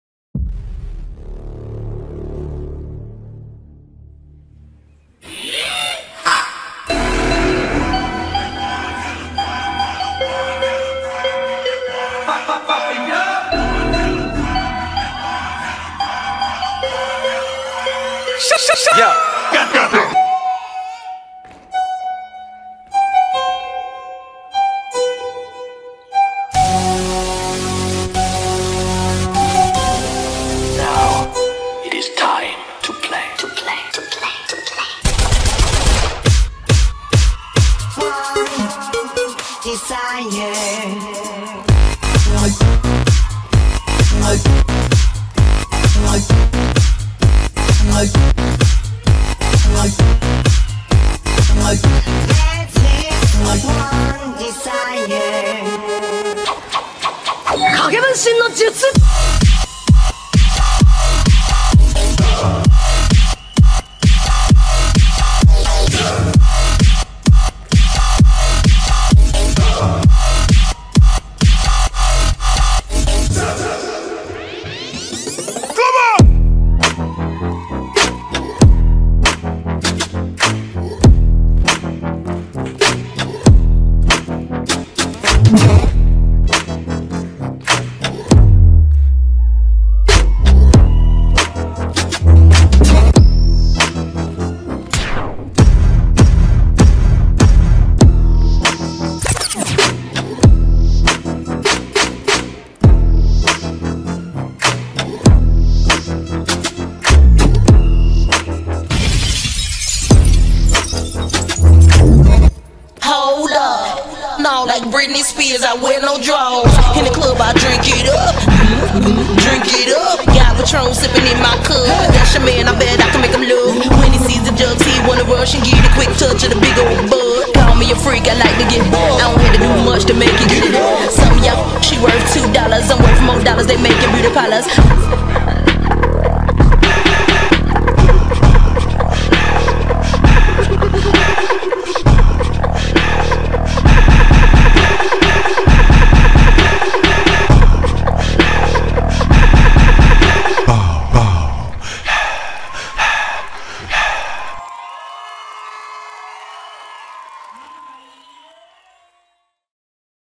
LIL STARKOS - 3 PLACE - Best dance SHOW kids BEGINNERS || WWDC CHAMP 15.04.23
Когда будет чемп:
🔸25-26 ноября 2023, Москва

Video by SMOKIN’ MONKEY CREATIVE & ELIN VIDEO